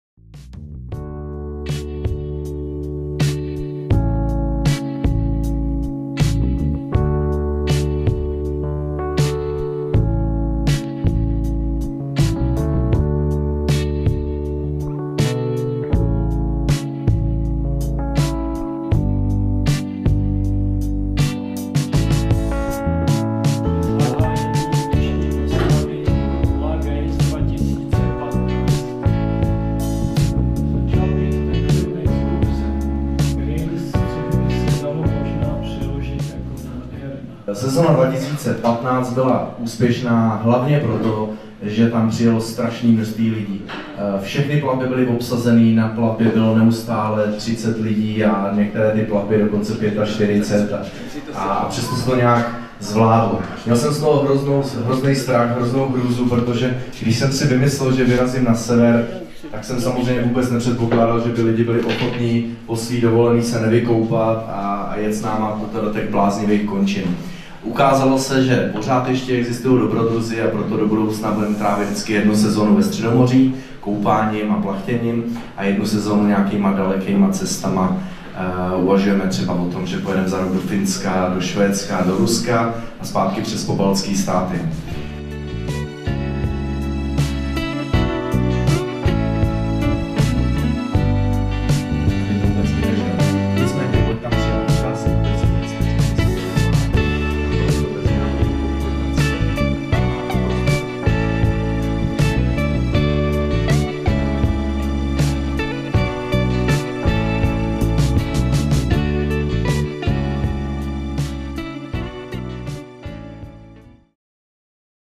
La Grace v Infocentru Hradec Králové
Vernisáž výstavy La Grace 2015 v královéhradeckém Infocentru  za přítomnosti kapitána lodi Josefa Dvorského...